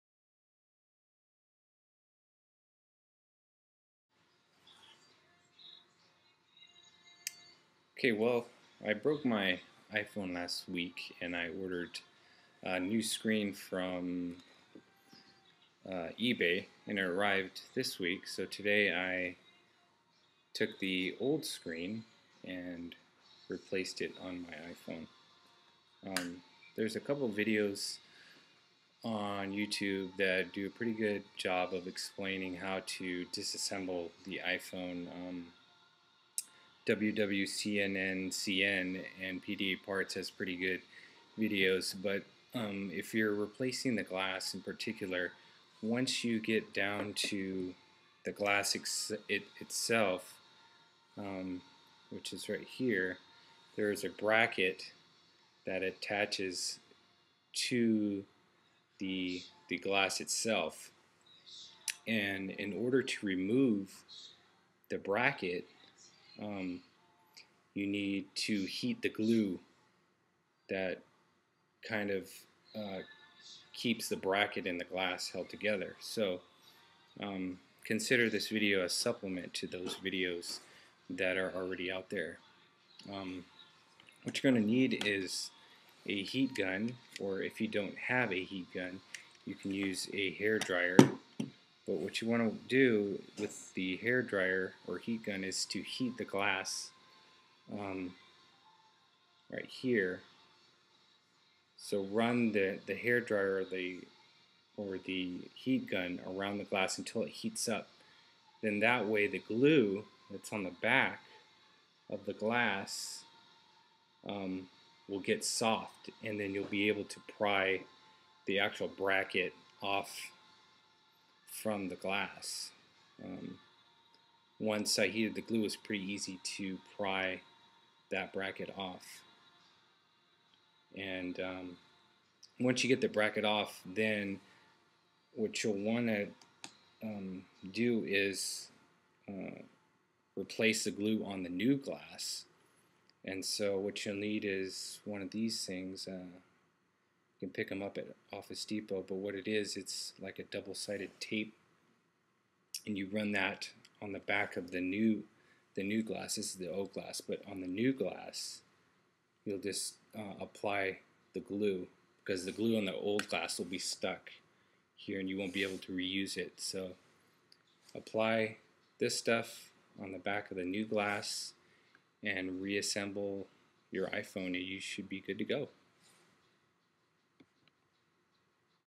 iphone 3G glass replacement repair
A supplement video explaining how to pry the bracket from the glass of the iPhone 3G.